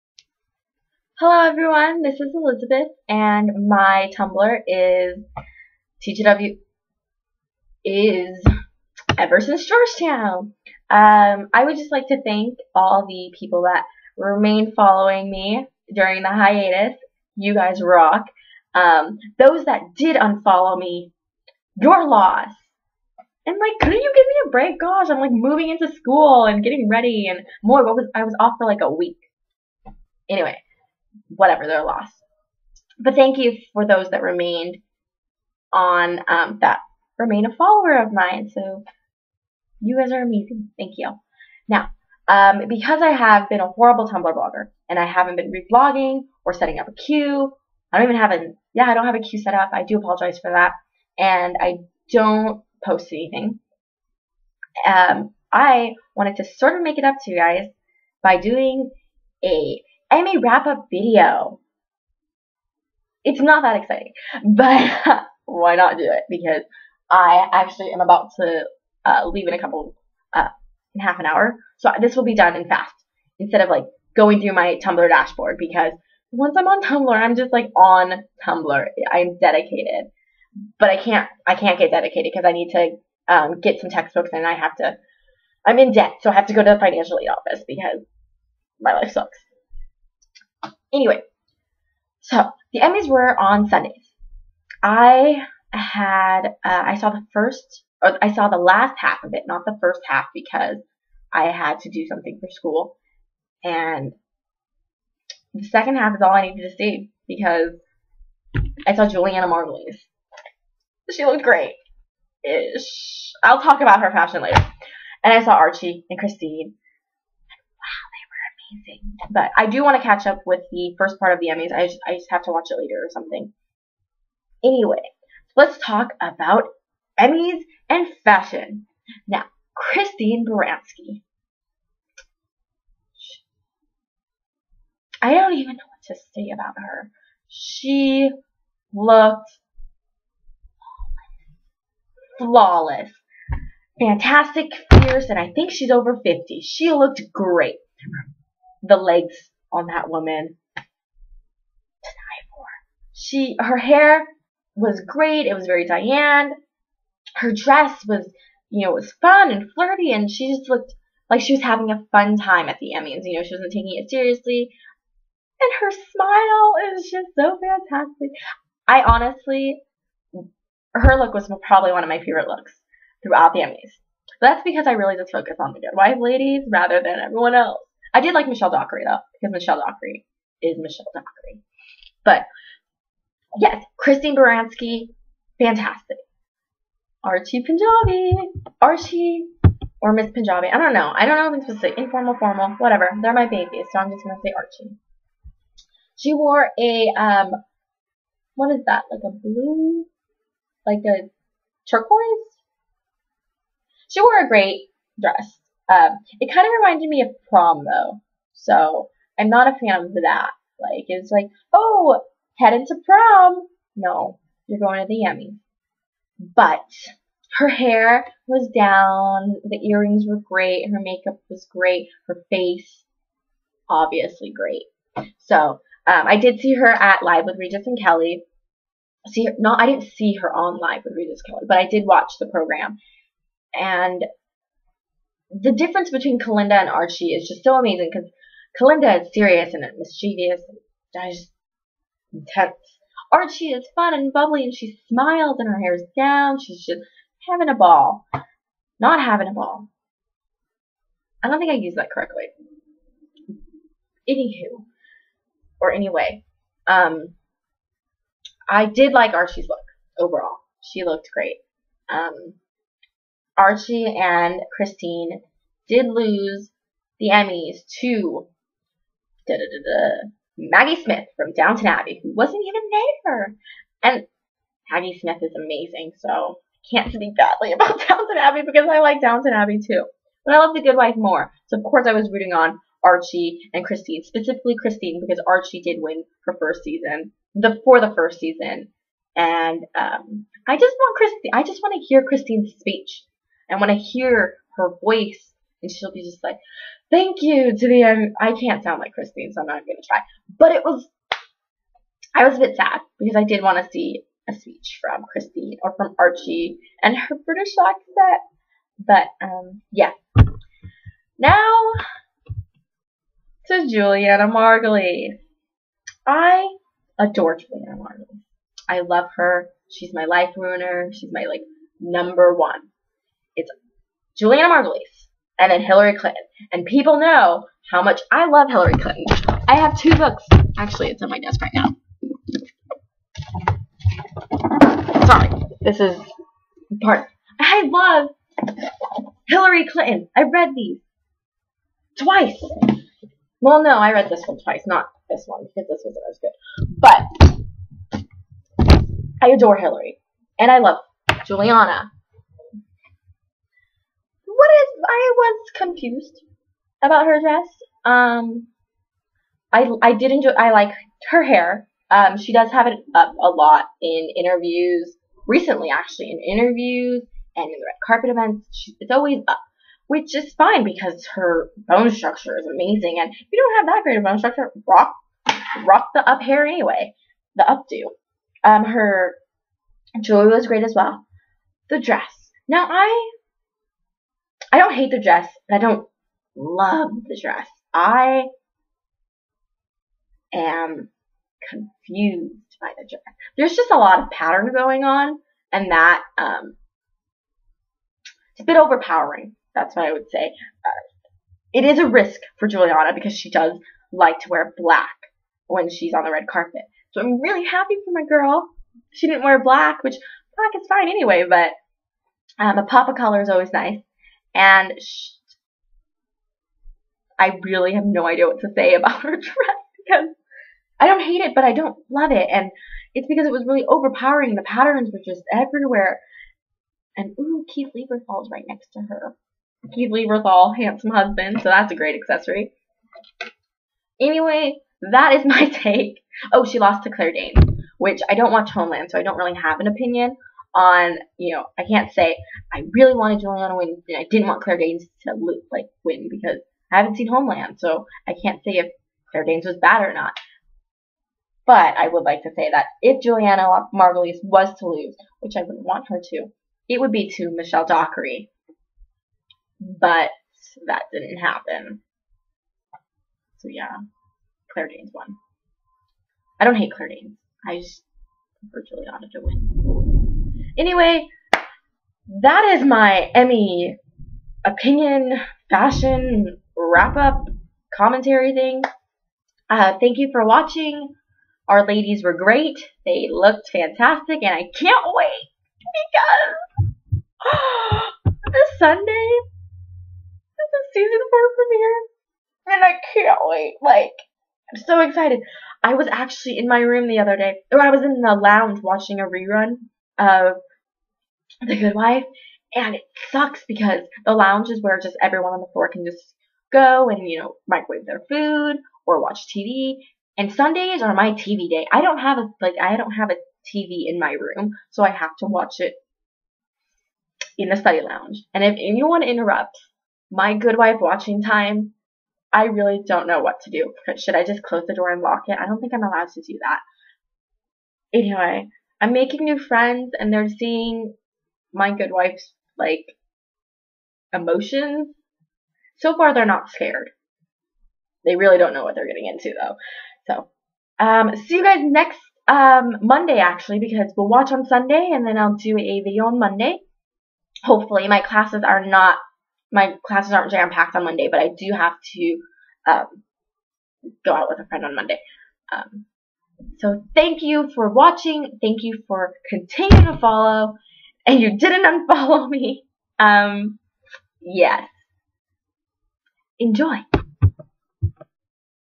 The Good Wife at the Emmys Commentary!
My opinion of the fashion looks of the ladies of The Good Wife at the Emmys :)